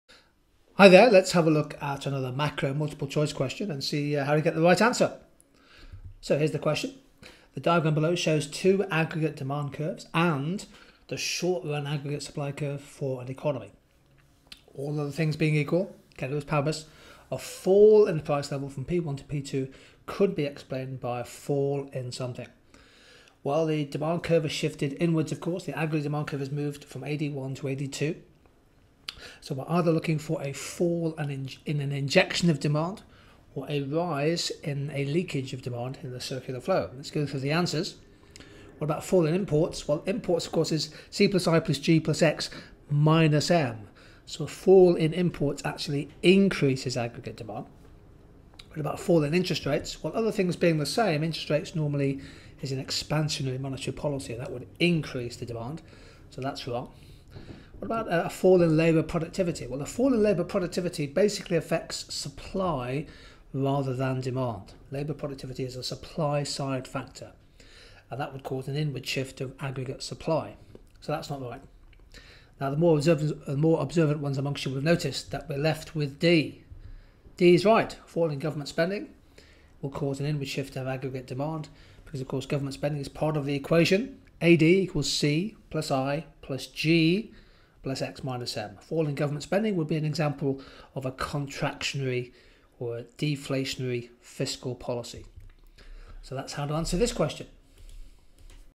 Macro Multi Choice: AD-AS Analysis
This question takes you through how to answer an AS macro multiple choice question on shifts in aggregate demand and supply